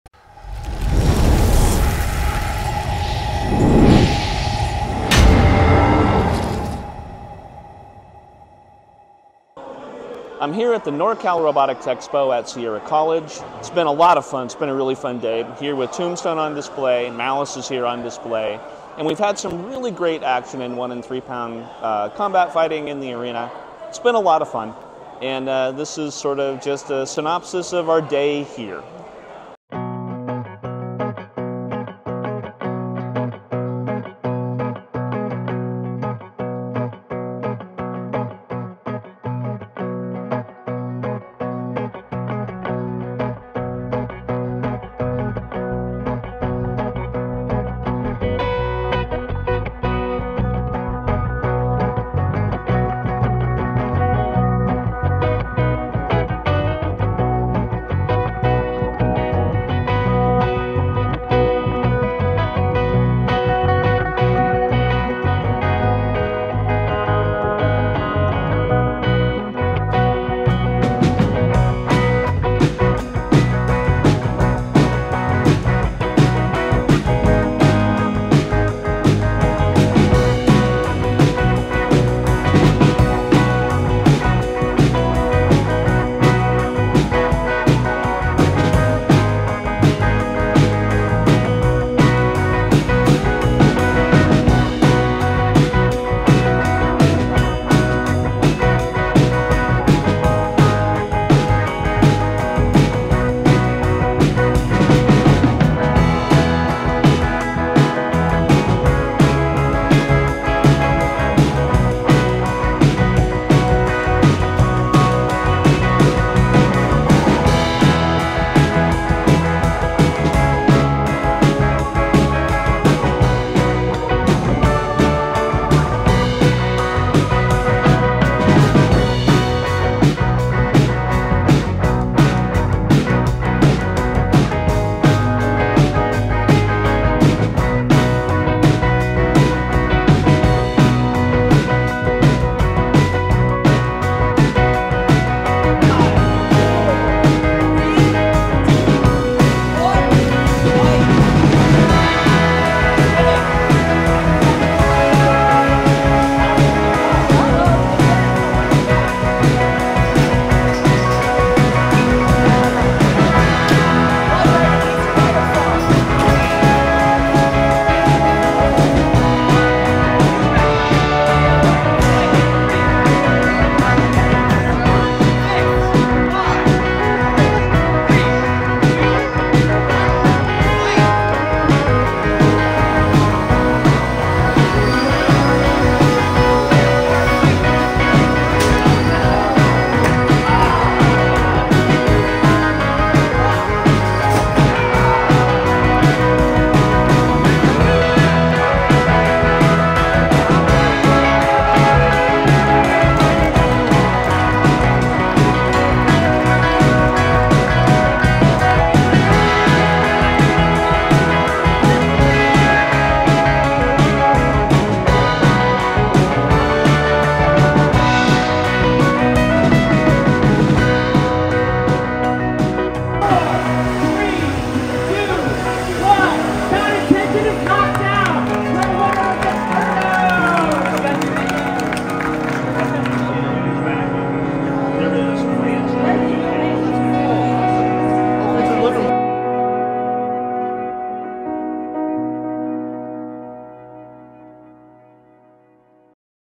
Tombstone on display at NorCal Robotics Expo 2023
Tombstone recently did a display at the Norcal Robotics expo at Sierra College in Rocklin. We were there, along with the Malice team, to help promote educational robotics in the area, and it was a lot of fun. This is an annual event, and we plan on returning for it next year too!

Team gear is now available in our web store, so go check it out if you're interested!